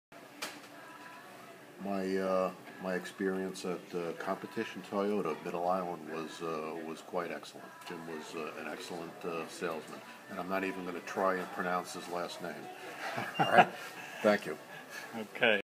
Competition Toyota Reviews: Testimonial by Norman about a 2014 Toyota Rav4
￼Meet Norman from Ridge NY, one of the many happy car buyers who bought a 2014 Toyota Rav4 at Competition Toyota! Make sure you stop by Competition Toyota today to take advantage of a huge inventory of ￼new and used vehicles.